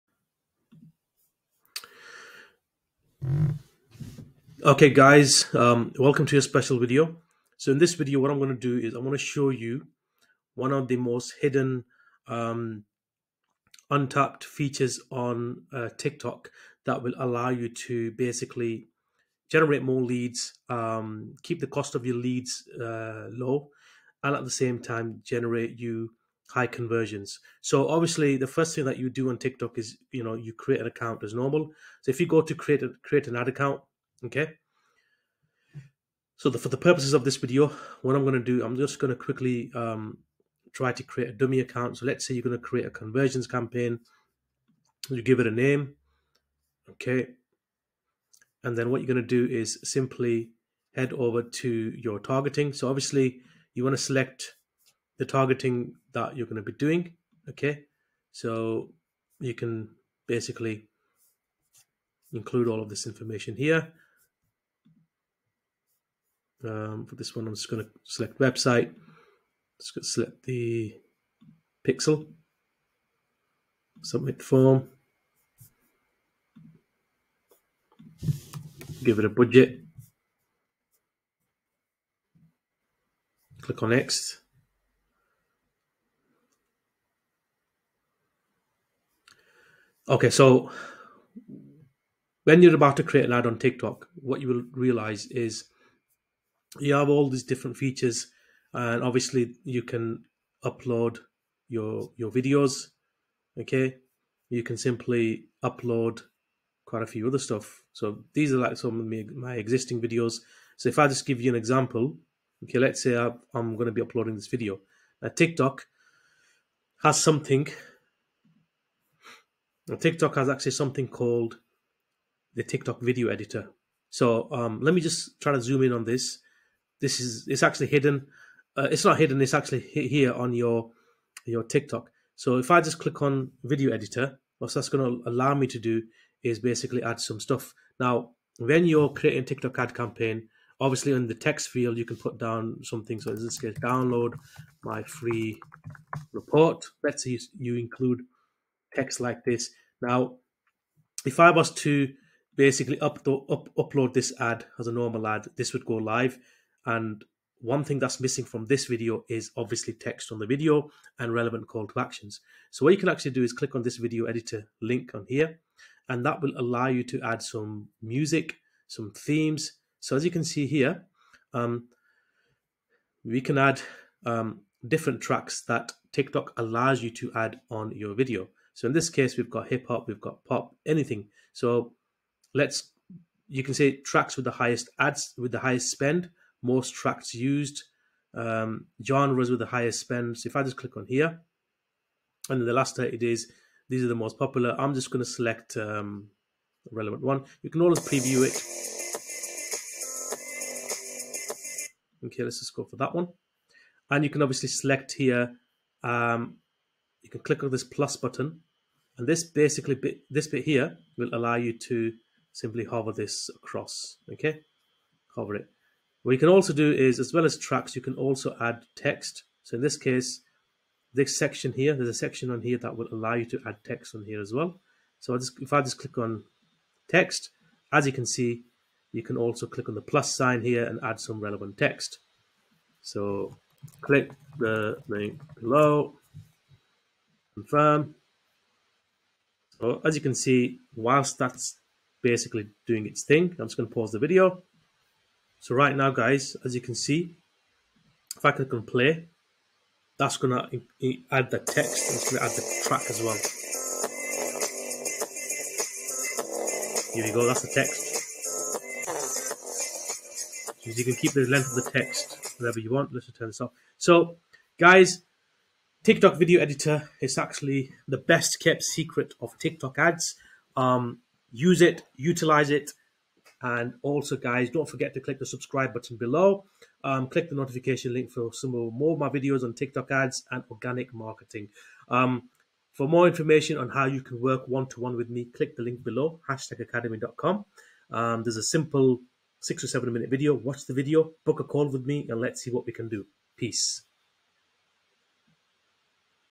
How To Get Cheap Leads From TikTok Ads Using THIS FREE Tool!
Learn how you can generate cheap leads using TikTok Ads using this FREE internal feature.